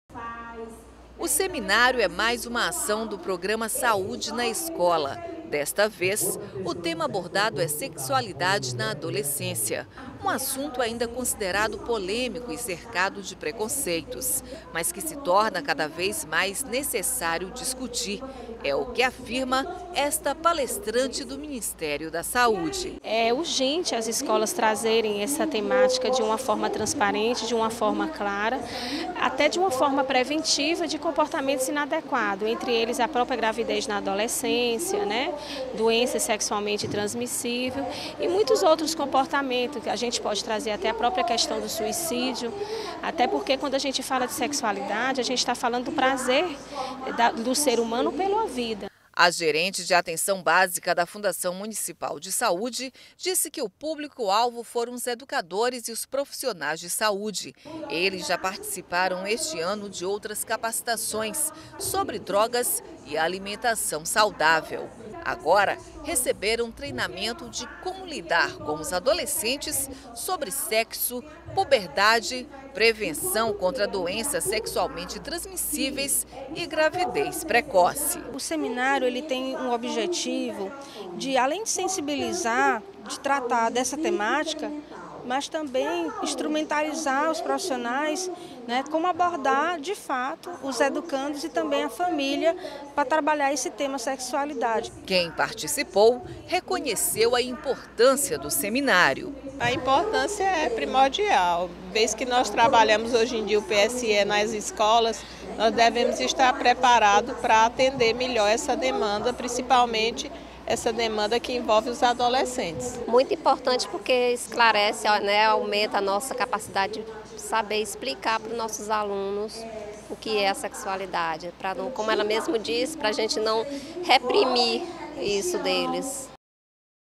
Sexualidade na escola é tema de seminário
Prevenção, informação, orientação. São temas tratados em seminário sobre sexualidade e educação destinado a educadores e profissionais de saúde. Com abordar esse tema na escola? Reportagem Elinor Campos.